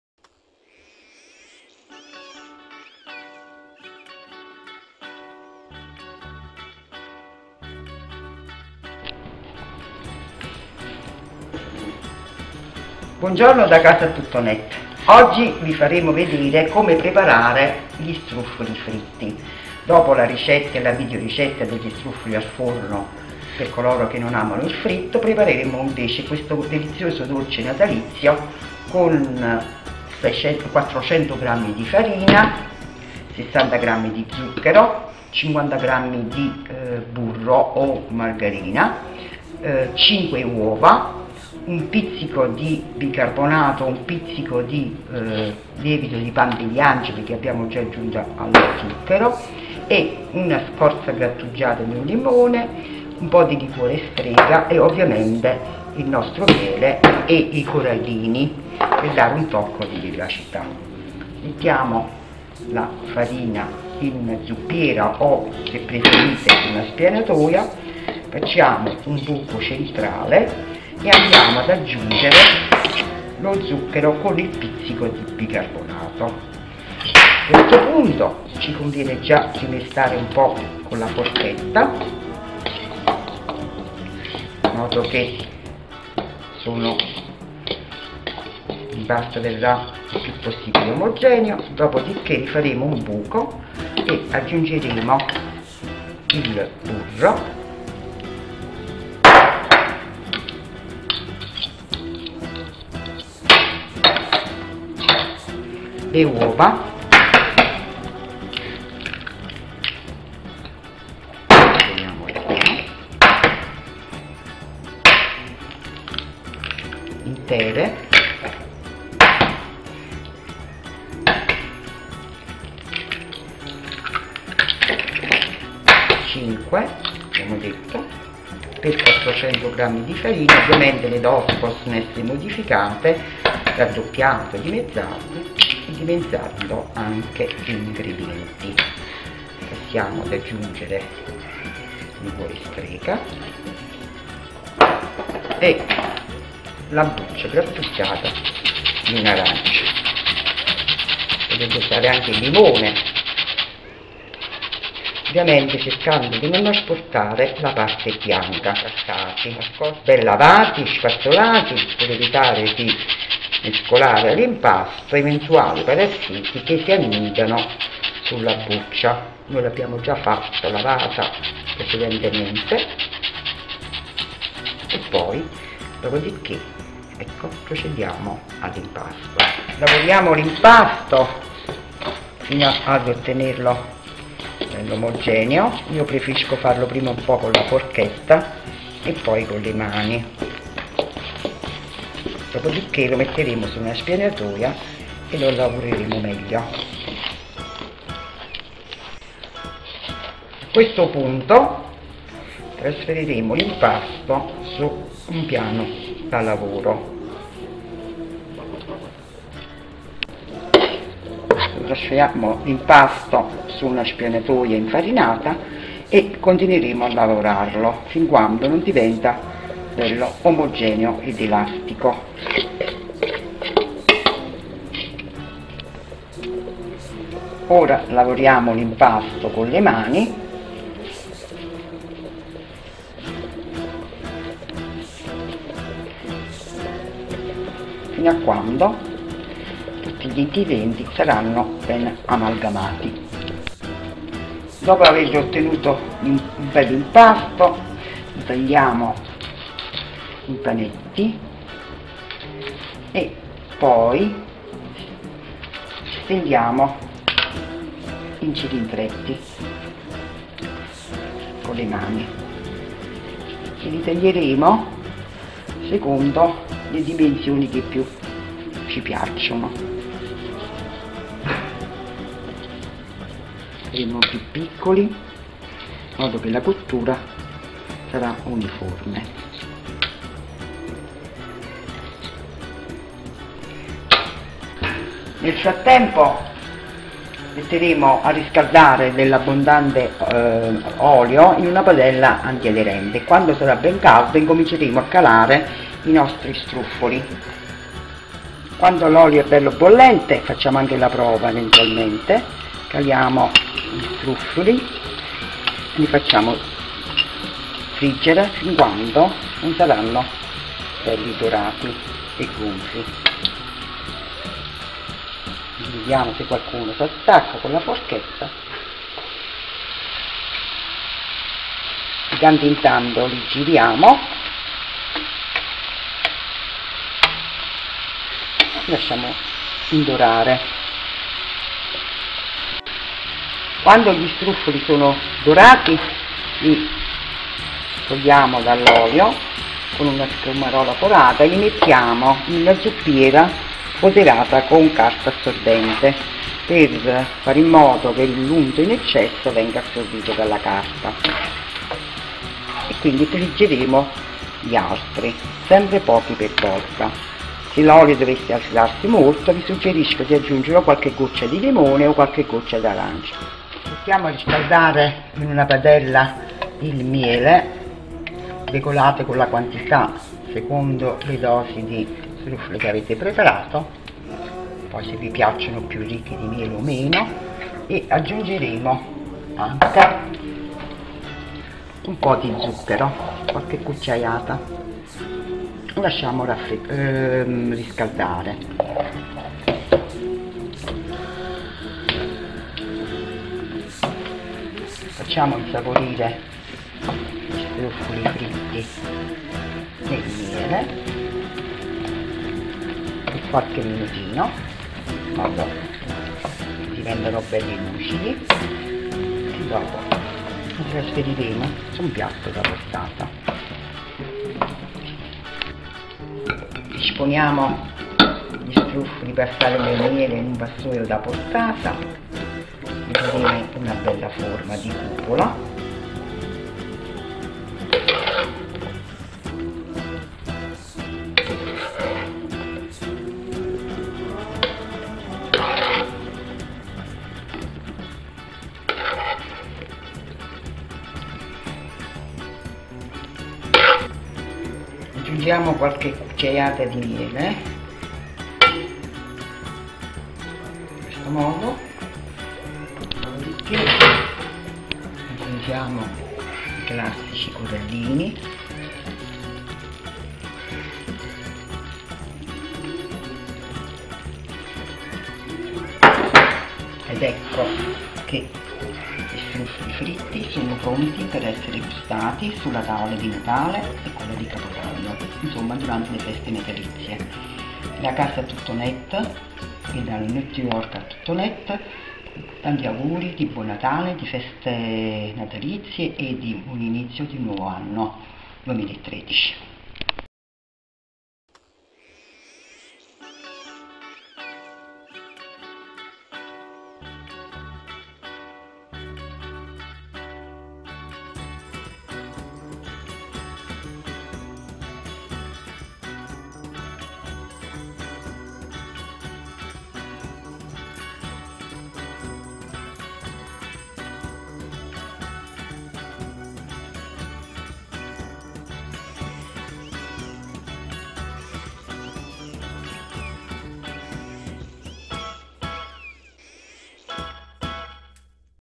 Ricetta struffoli fritti - casaegiardino.it
Come preparare gli struffoli : dall'imlpasto alla cottura
Formate con la farina setacciata una  fontana, mettete le uova intere, il burro, lo zucchero, il bicchierino di liquore, la bustina di zuccchero vanigliato, il sale, il bicarbonato, la buccia di arancia o limone grattugiata ed impastate.
Lavorate l'impasto fino a quando non si stacca dalle mani, poi formate dei bastoncini cilindrici e tagliateli a piccoli pezzi.